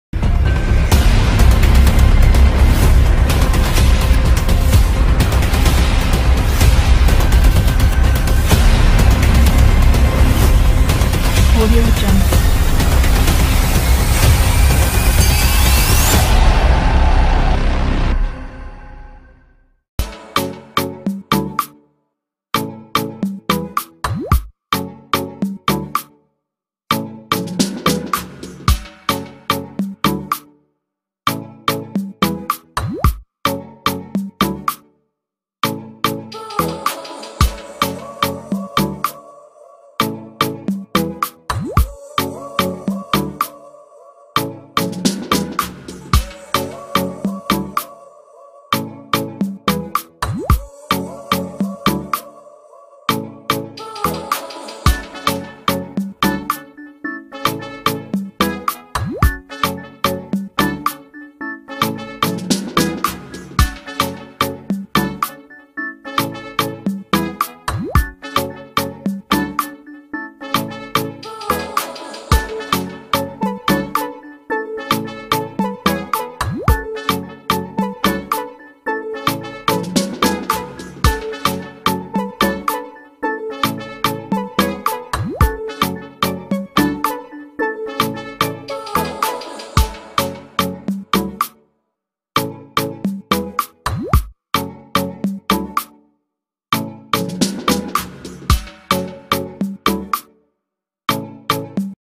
TOUS LES RESULTATS DES QUARTS DE FINALE DE LA CAN 2021
DÉCOUVRONS ENSEMBLE DANS CETTE VIDÉO TOUS LES RESULTATS DES QUARTS DE FINALE DE LA CAN 2021

NOUVELLE VIDÉO TOUS LES WEEK-END

📝 ECRITURE : DSM32

🖥 Montage by DSM32

🎙️Voix Off by DSM32

🖥 MONTAGE VIDEO : DSM32 CONCEPTION

▶️Lien de la chaine youtube :

© Copyright DSM32